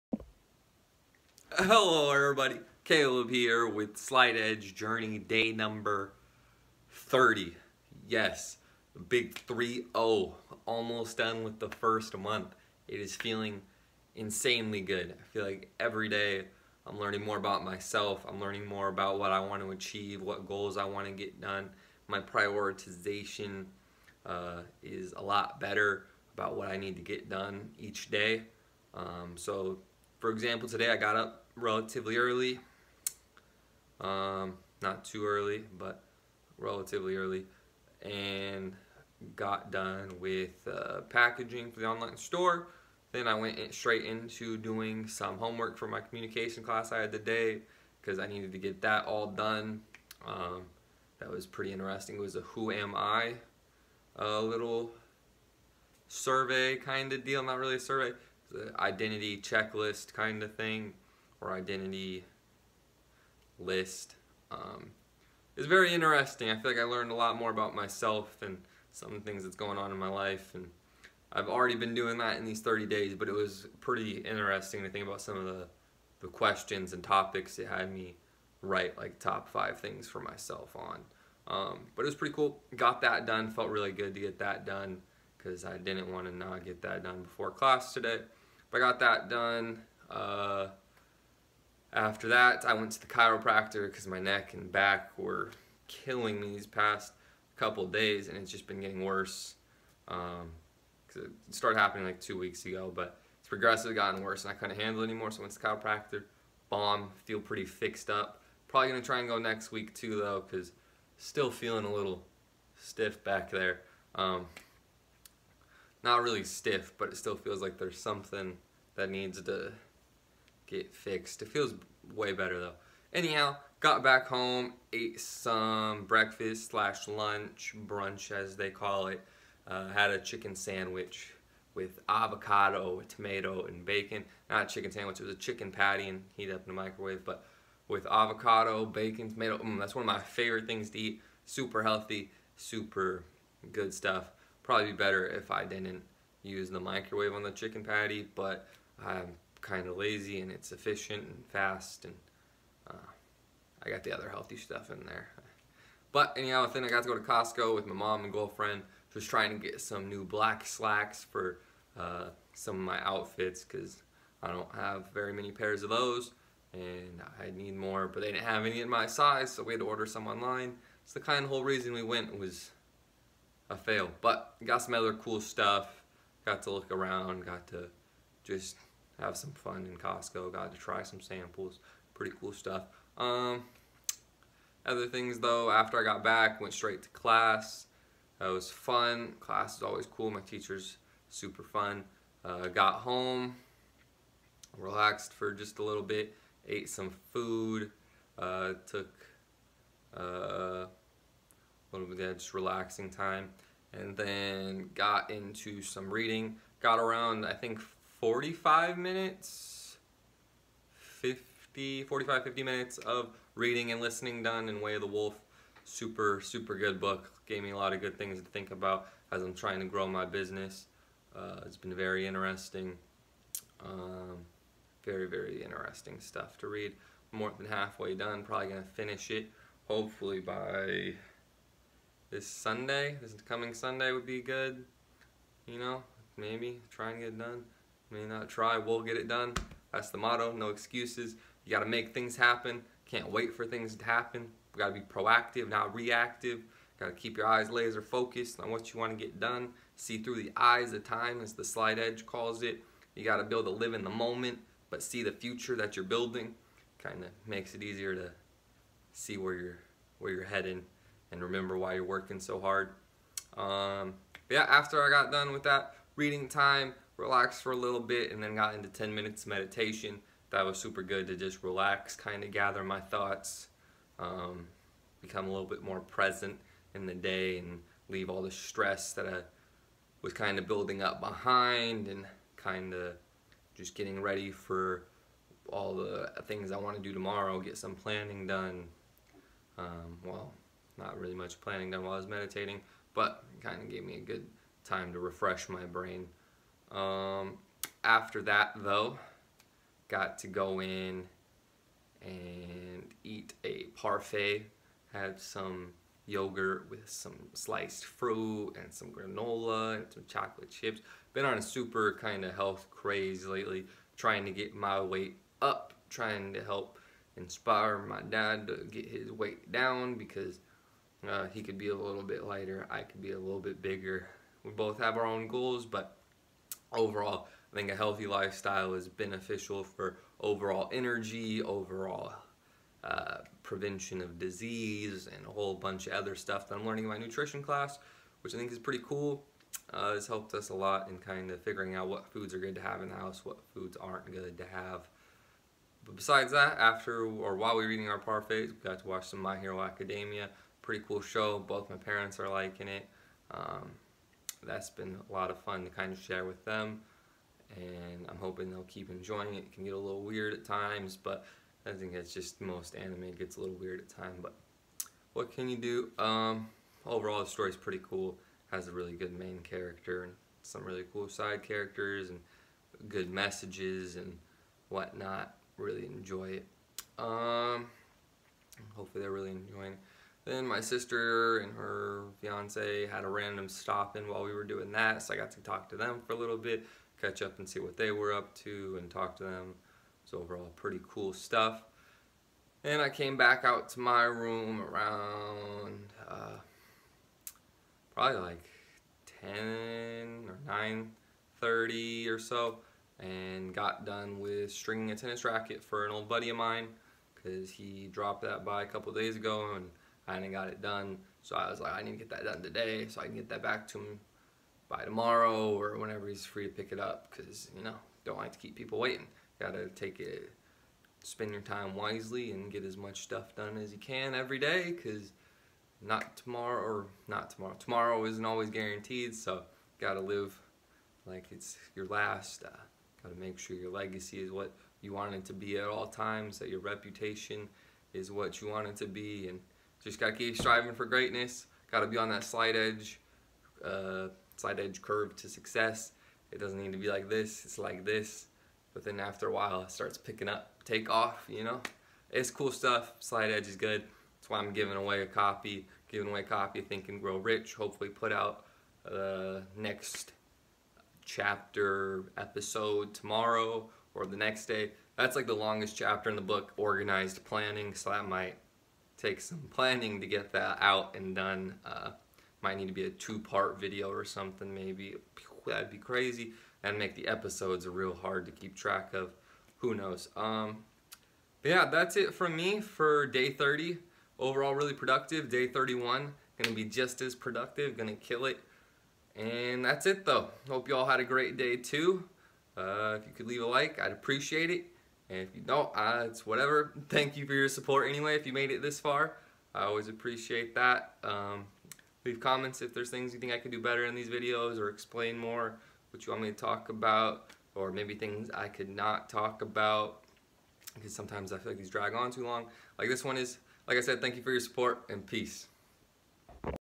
Sheesh - Slight Edge Journey Day #30
Y’all ready know the deal. I hope y’all had a good and productive day. Thank you for your support and I’ll see you tomorrow at the end of the first month!